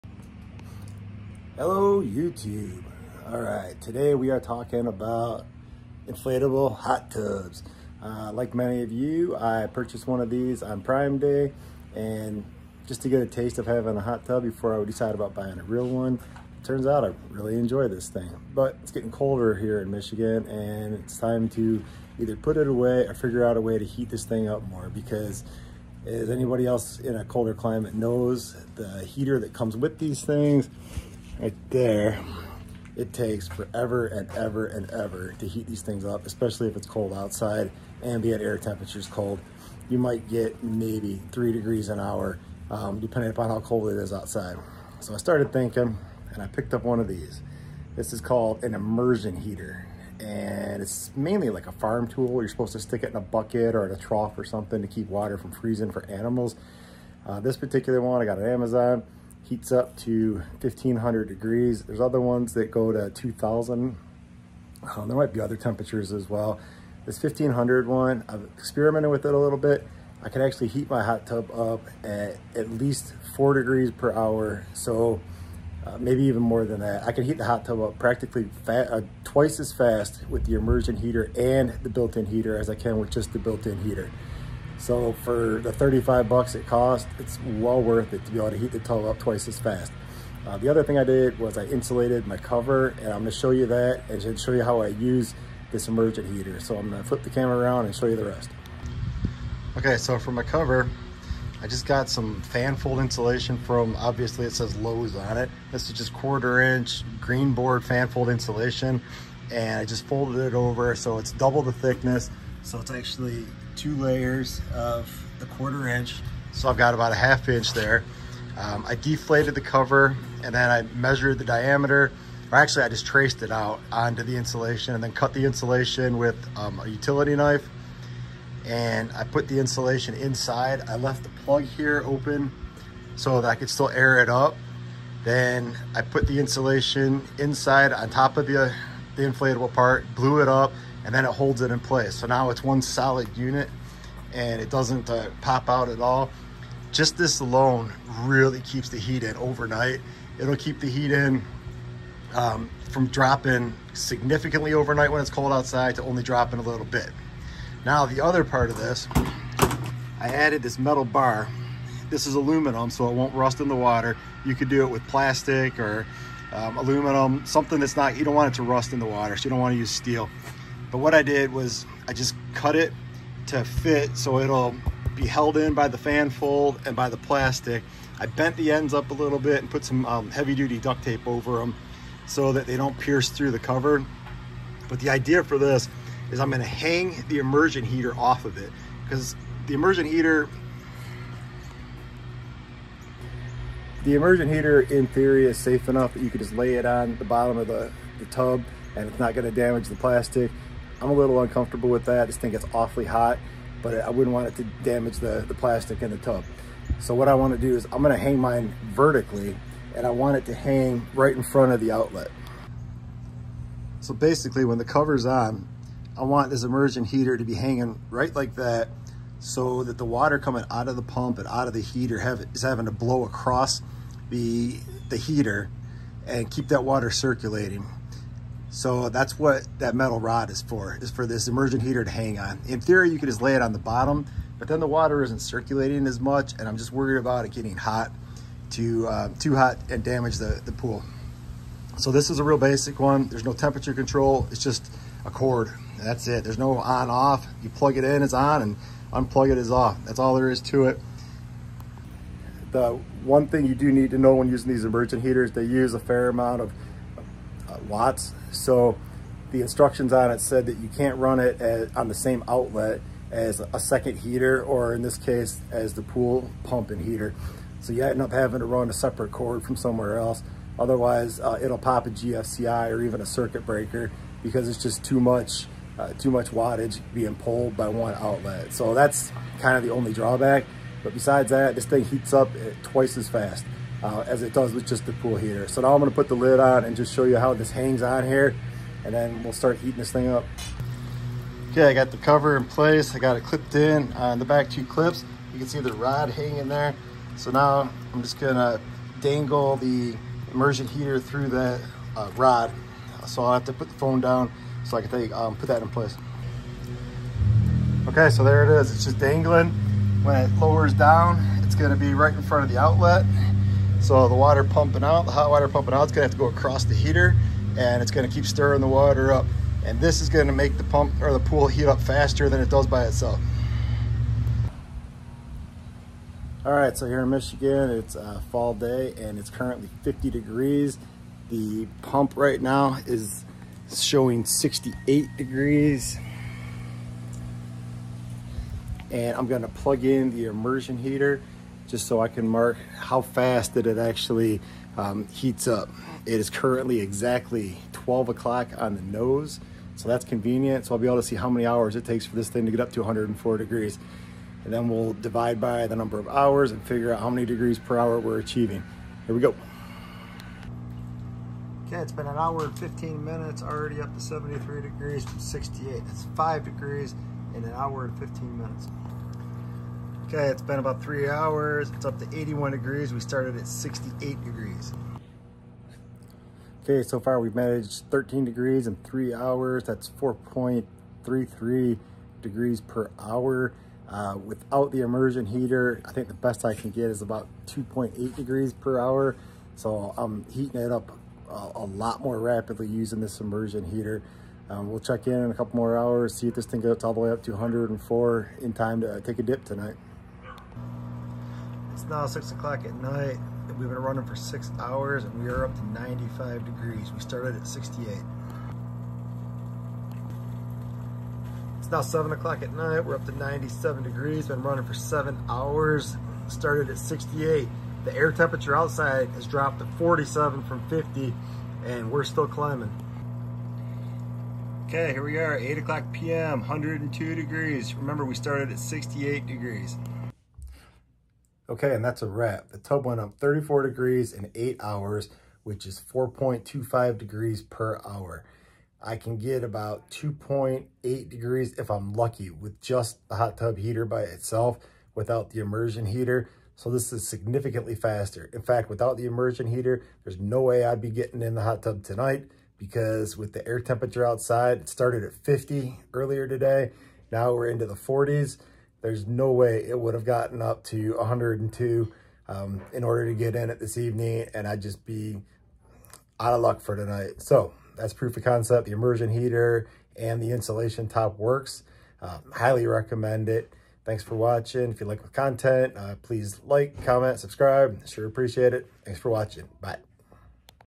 Inflatable Hot Tub Hacks - Immersion Heater and Insulated cover
In my video I made a mistake saying the heater was 1500 degrees.  It’s actually 1500 watts.  I misspoke and didn’t want to shoot the video again.

This video shows how I mount and use an immersion heater with my inflatable hot tub / spa.  I also added insulation / fanfold to the cover which helps retain heat as well.

I can get 4.25 degrees per hour using the immersion heater.